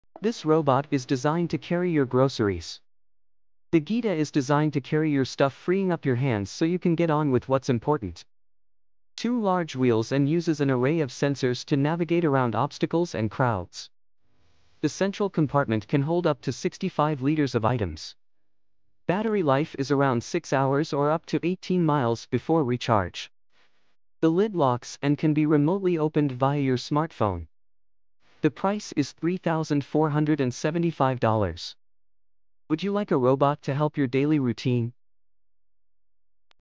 Would you like a robot to help your daily routine?
This robot is designed to carry your groceries. @mavigadgetoriginals

The Gita is designed to carry your stuff freeing up your hands so you can get on with what’s important.

Two large wheels and uses an array of sensors to navigate around obstacles and crowds.

The central compartment can hold up to 65 liters of items.

Battery life is around six hours or up to 18 miles before recharge.

The lid locks and can be remotely opened via your smartphone.

The price is $3,475.